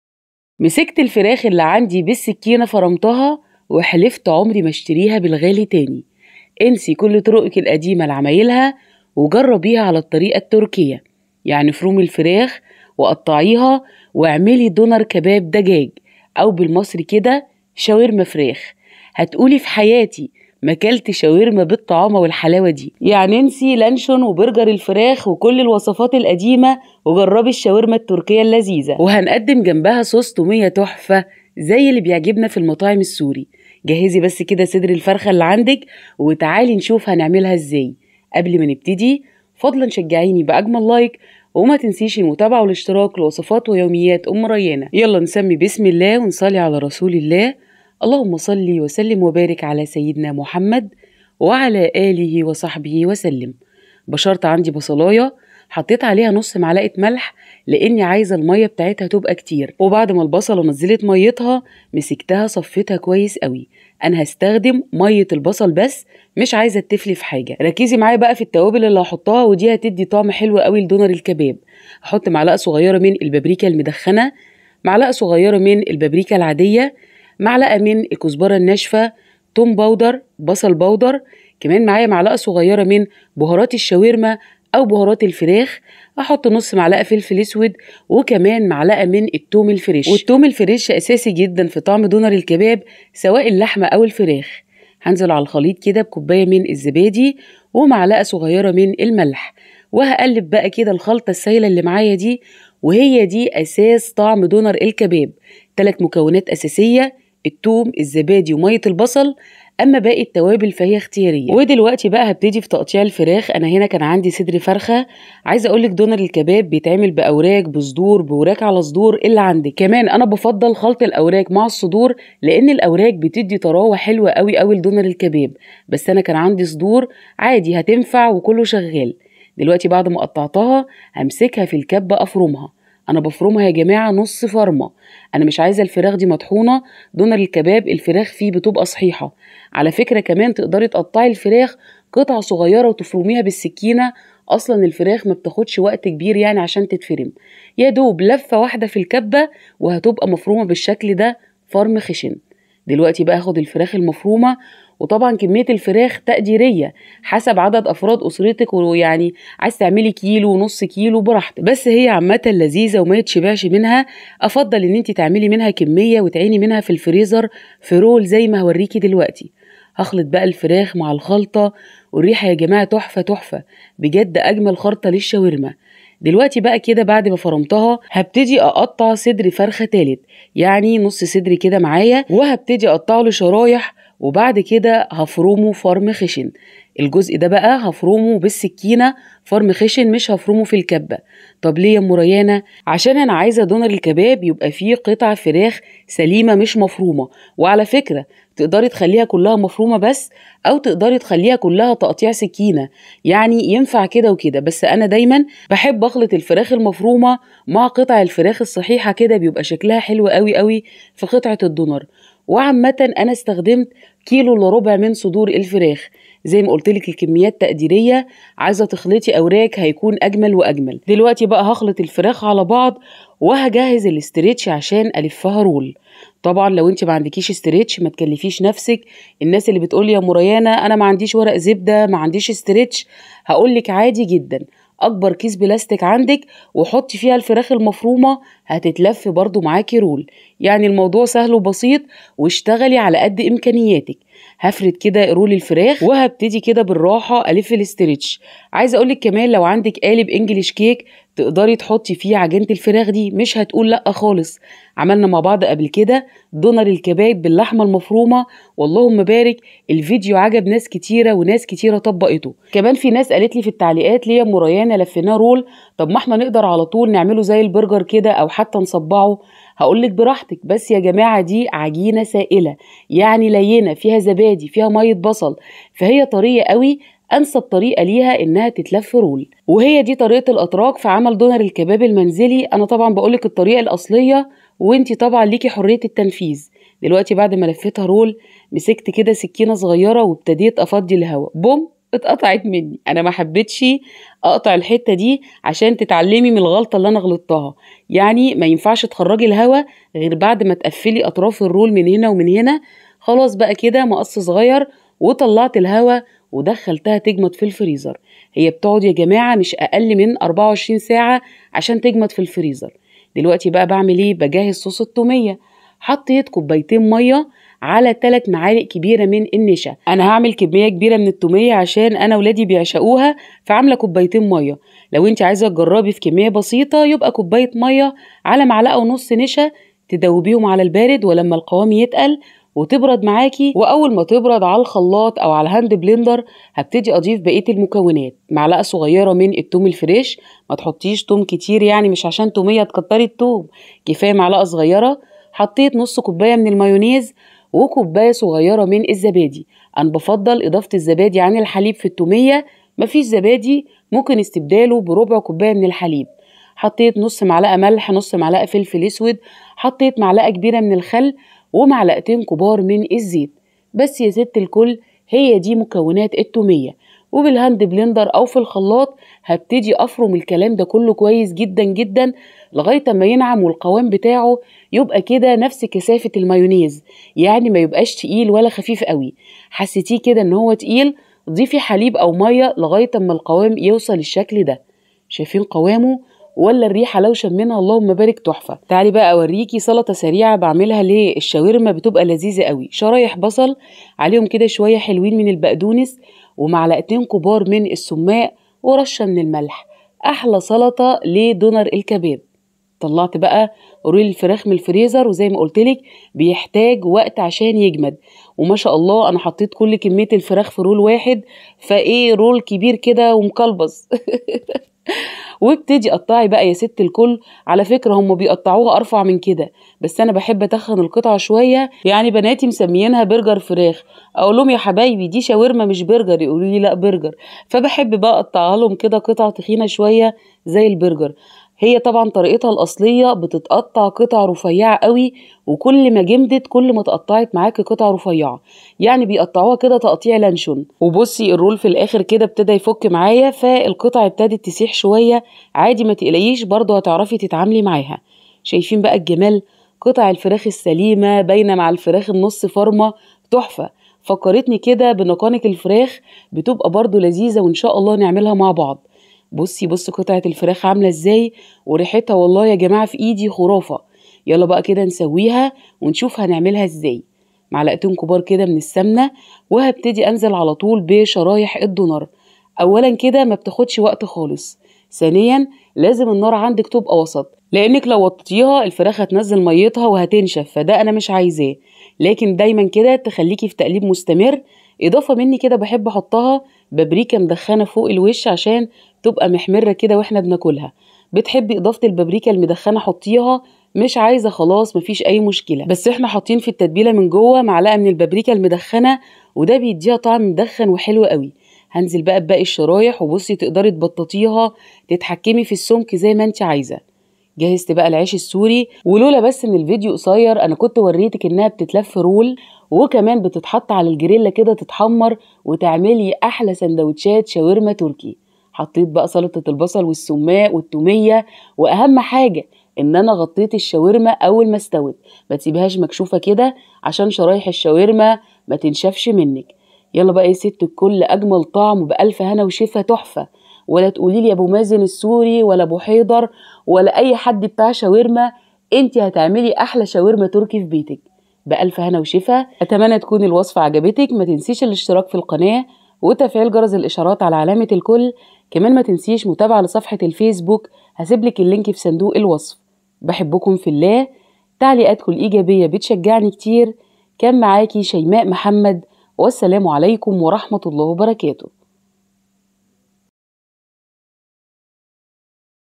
فرمت الفراخ بالسكينة وحمرتها حلفت عمرى ما اشتريها قال واقفين طوابير عليها دونر كباب دجاج انسى القديم
شاورما الدجاج التركية احلى واجمل طريقه لدونر الكباب التركى خطيره صوص ثوميه الذ طعم